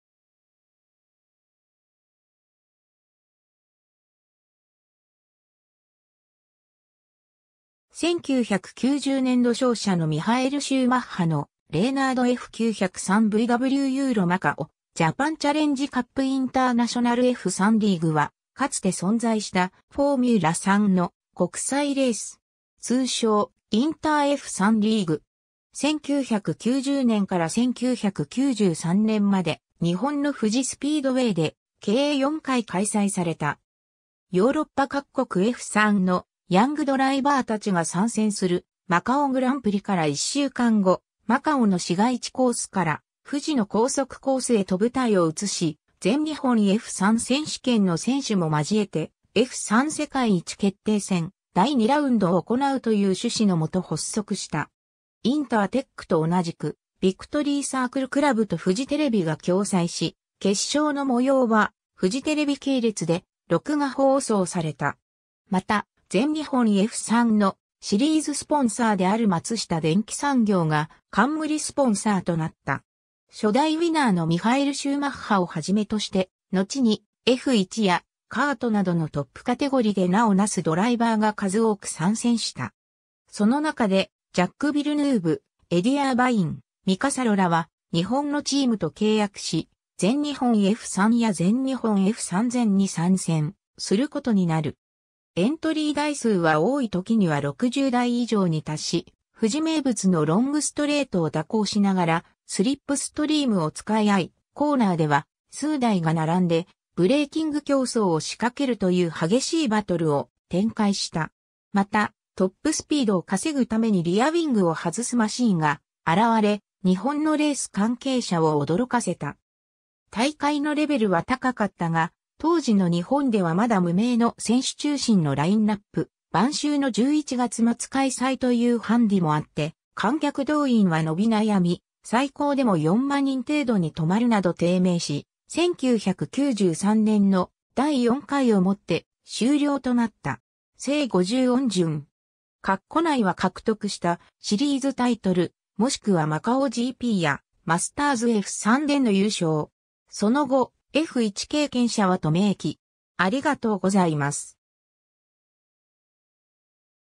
インターナショナルF3リーグ
1990年度勝者のミハエル・シューマッハのレイナードF903・VW ユーロ-マカオ-ジャパン チャレンジカップ インターナショナルF3リーグ（EURO-MACAU-JAPAN CHALLENGE CUP INTERNATIONAL F3 LEAGUE ）は、かつて存在したフォーミュラ3 (F3) の国際レース。
通称「インターF3リーグ」。
1990年から1993年まで日本の富士スピードウェイで計4回開催された。
 ヨーロッパ各国F3のヤングドライバー達が参戦するマカオグランプリから1週間後、マカオの市街地コースから富士の高速コースへと舞台を移し、全日本F3選手権の選手も交えて「F3世界一決定戦第2ラウンド」を行うという趣旨のもと発足した。
インターTECと同じくビクトリー・サークルクラブ (VICIC) とフジテレビが共催し、決勝の模様はフジテレビ系列で録画放送された。
また、全日本F3のシリーズスポンサーである松下電器産業（パナソニック）が冠スポンサーとなった。
 初代ウィナーのミハエル・シューマッハを始めとして、のちにF1やCARTなどのトップカテゴリで名を成すドライバーが数多く参戦した。
その中でジャック・ヴィルヌーヴ、エディ・アーバイン、ミカ・サロらは日本のチームと契約し、全日本F3や全日本F3000に参戦することになる。
 エントリー台数は多い時には60台以上に達し、富士名物のロングストレートを蛇行しながらスリップストリームを使い合い、コーナーでは数台が並んでブレーキング競争を仕掛けるという激しいバトルを展開した。
また、トップスピードを稼ぐためにリアウィングを外すマシンが現れ、日本のレース関係者を驚かせた。
大会のレベルは高かったが、当時の日本ではまだ無名の選手中心のラインナップ、晩秋の11月末開催というハンディもあって観客動員は伸び悩み、最高でも4万人程度に留まるなど低迷し、1993年の第4回をもって終了となった。
 姓50音順。
括弧内は獲得したシリーズタイトル、もしくはマカオGPやマスターズF3での優勝。
その後F1経験者は（F1）と明記。